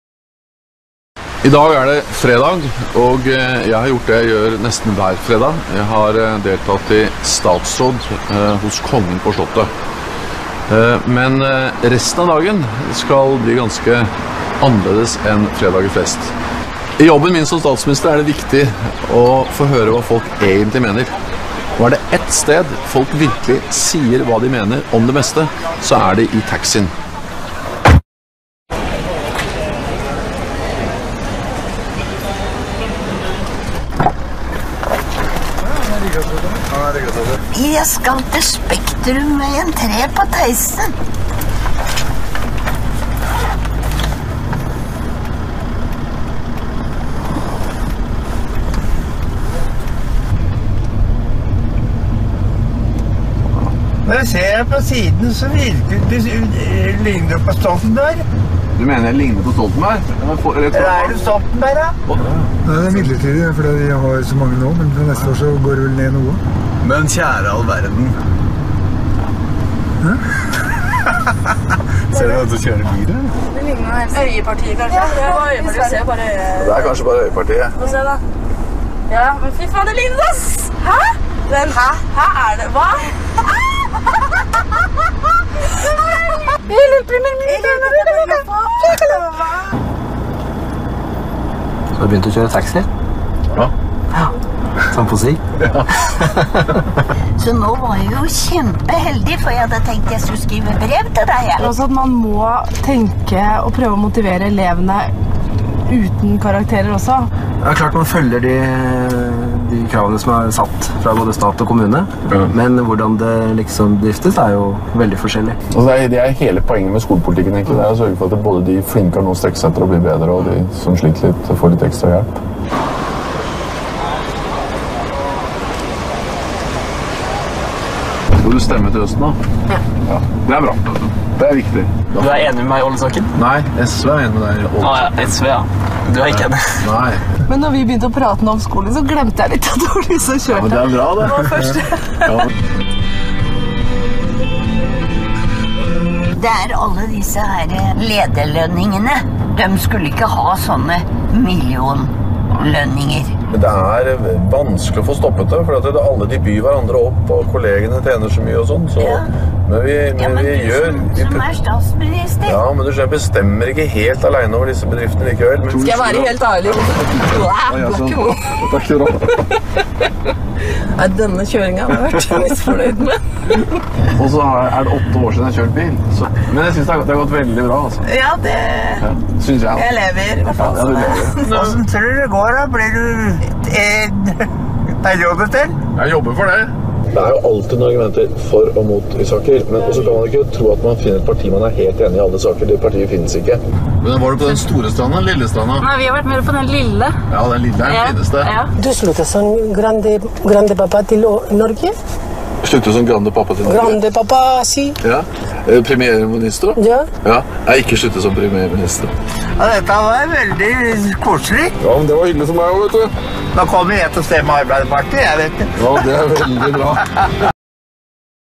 Taxi Stoltenberg - Norway's PM Jens Stoltenberg turns taxi driver to woo voters
Taxi Stoltenberg.

Jens Stoltenberg som taxisjåfør i skjult kamera-video omtales bredt utenfor Norges grenser i dag.
«Den norske statsministeren Jens Stoltenberg jobbet en ettermiddag i skjul som taxisjåfør i Oslo».

Labour leader says he wanted to hear what passengers really thought by driving a taxi around Oslo for several hours.
Norway's prime minister worked secretly as a taxi driver in central Oslo for a day in June, leaving his passengers wondering whether their elected leader had quit the day job.
Wearing a taxi driver's uniform and sunglasses, Jens Stoltenberg drove passengers around the streets of the Norwegian capital for several hours, confirming his identity only after his passengers realised who he was.

Norwegens Premier Stoltenberg inkognito als Taxifahrer unterwegs.
Regierungschef stellte mit versteckter Kamera gefilmtes Video vor: "Es ist wichtig für mich zu hören, was die Leute wirklich denken".